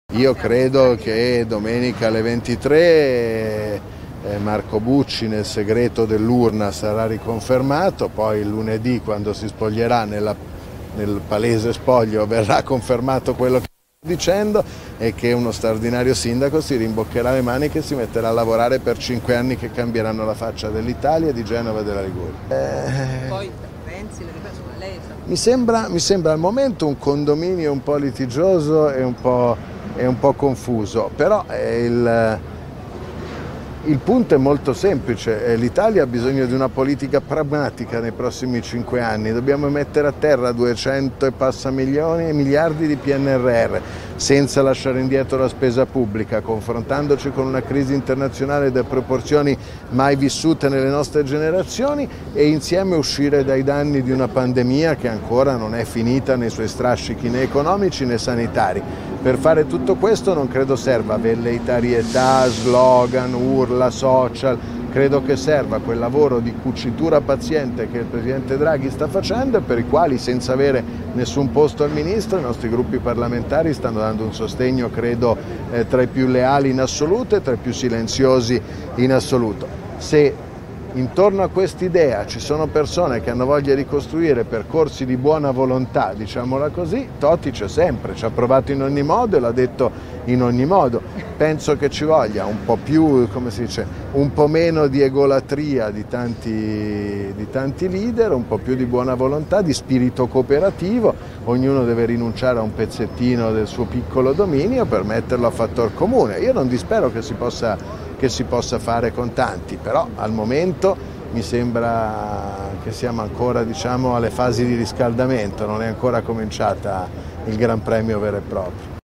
Governo, Toti: "Parlamento condominio litigioso ma bisogna costruire nuova Italia post pandemia"
"Marco Bucci verrà riconfermato sindaco di Genova senza dubbi"

POLITICA (Roma). "Credo che domenica alle 23 Marco Bucci sarà riconfermato sindaco di Genova. Sembra che al momento il Parlamento adesso sia un condominio litigioso e confuso, l'Italia però ha bisogno di una politica pragmatica per mettere in campo nei prossimi 5 anni i progetti che riporteranno alla ripresa del Paese dopo le conseguenze drammatiche della pandemia. Il Presidente Mario Draghi credo debba continuare il suo lavoro, serve buona volontà da parte di tutte le parti politiche di costruire insieme il futuro del Paese. Siamo alle fasi iniziali comunque" commenta ancora Giovanni Toti a Chigi. (Vanessa Ciccarelli/alanews)

Io credo che è domenica alle 23 e Marco Bucci nel Segreto dell'urna sarà riconfermato poi lunedì quando si toglierà nella Palese voglio verrà confermato quello dicendo è che uno straordinario sindaco si rimbocca le maniche si metterà a lavorare per 5 anni che cambieranno la faccia dell'Italia di Genova e della Liguria mi sembra mi sembra il momento un condominio è un po' litigioso è un po' è un po' confuso però è il il punto è molto semplice e l'Italia ha bisogno di una politica pragmatica nei prossimi cinque anni dobbiamo mettere a terra 200 e passa milioni e miliardi di PNR Senza lasciare indietro spesa pubblica confrontandoci con una crisi Internazionale delle proporzioni mai vissute nelle nostre generazioni e insieme uscire dai danni di una pandemia che ancora non è finita nei suoi strascichi economici nei sanitari per fare tutto questo non credo serva velleitarie età slogan urla Social credo che serva quel lavoro di cucitura paziente che è presente draghi sta facendo per i quali senza avere nessun posto al ministro nostri gruppi parlamentari sta andando un sostegno credo è tra i più leali In assoluto e tre più silenziosi in assoluto Se intorno quest'idea ci sono persone che hanno voglia di costruire percorsi di buona volontà diciamola così Totti c'ho sempre ci ho provato in ogni modo l'ha detto in ogni modo penso che ci voglia un po' più un po' meno di egolatria di tanti di tanti L'idea era un po' più di buona volontà di spirito cooperativa ognuno deve rinunciare un pezzettino del suo piccolo dominio permette ha fatto il comune io non ti Spero che si possa che si possa fare con tanti però al momento mi sembra che siamo ancora diciamo alle fasi di riscaldamento non è ancora cominciata il Gran Premio vero e proprio